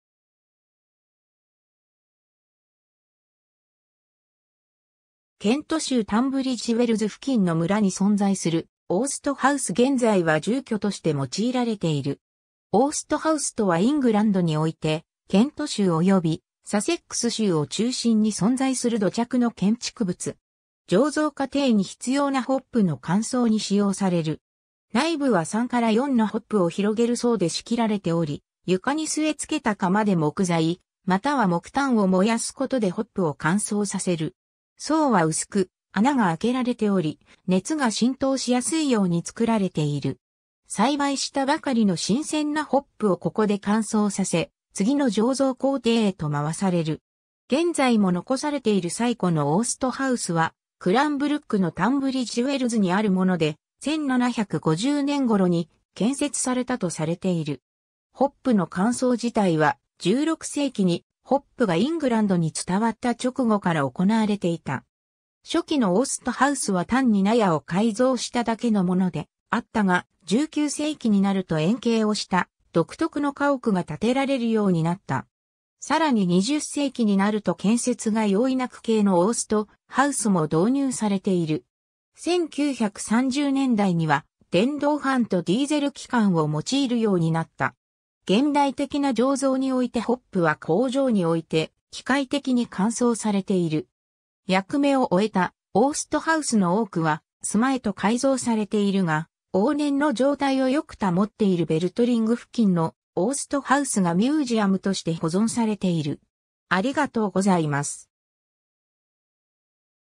オースト・ハウス
ケント州タンブリッジ・ウェルズ付近の村に存在するオースト・ハウス 現在は住居として用いられている。
 オースト・ハウス (oast house) とはイングランドにおいてケント州およびサセックス州を中心に存在する土着の建築物。
醸造過程に必要なホップの乾燥に使用される。
内部は3から4のホップを広げる層で仕切られており、床に据え付けた窯で木材または木炭を燃やすことでホップを乾燥させる。
層は薄く穴があけられており、熱が浸透しやすいように作られている。
栽培したばかりの新鮮なホップをここで乾燥させ、次の醸造工程へとまわされる。
 現在も残されている最古のオースト・ハウスはクランブルックのタンブリッジ・ウェルズにあるもので、1750年頃に建設されたとされている。
ホップの乾燥自体は、16世紀にホップがイングランドに伝わった直後から行われていた。
初期のオースト・ハウスは単に納屋を改造しただけのものであったが、19世紀になると円形をした独特の家屋が建てられるようになった。
さらに20世紀になると建設が容易な矩形のオースト・ハウスも導入されている。
1930年代には電動ファンとディーゼル機関を用いるようになった。
 現代的な醸造においてホップは工場において機械的に乾燥されている。
役目を終えたオースト・ハウスの多くは住居へと改造されているが、往年の状態を良く保っているベルトリング付近のオースト・ハウスがミュージアムとして保存されている。